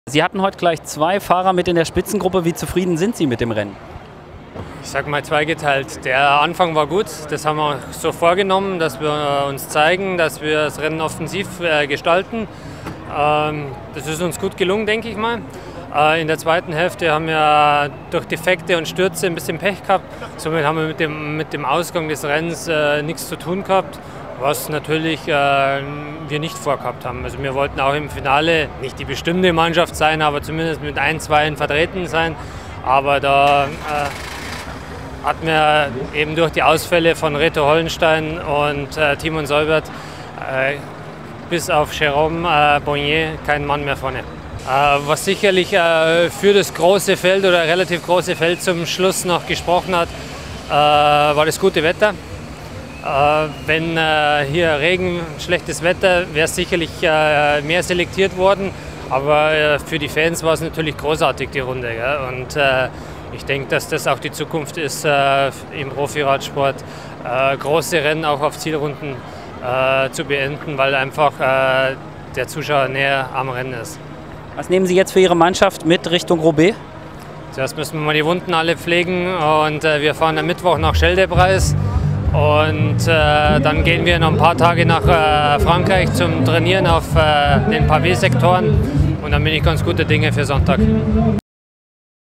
Flandernrundfahrt 2012: NetApp Team Manager Ralph Denk im Interview
Das Team NetApp war heute mit gleich 2 Fahrern in der Fluchtgruppe vertreten. Ob Team Manager Ralph Denk mit der Leistung seines Teams zufrieden ist und was er über die neue Strecke denkt -- im Video.